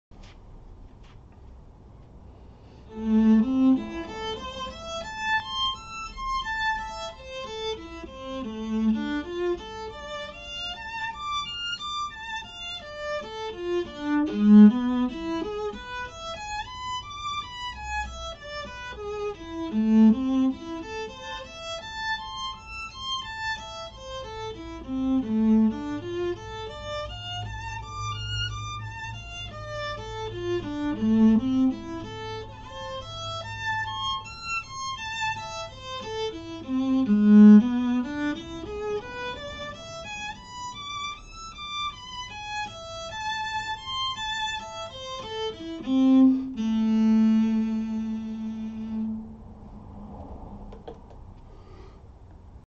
Practice Challenge Day 18 of 30. Arpeggios.
This challenge is hard. Yesterday was such a major fail (today not great either). So depressed right now. Very emo. Sad times :( I mean, who even cares haha! Just trying to hang with it. Why in the world would a talentless 58 year old beginner be compelled to continue? Puzzling. I'm a mystery wrapped in a conundrum.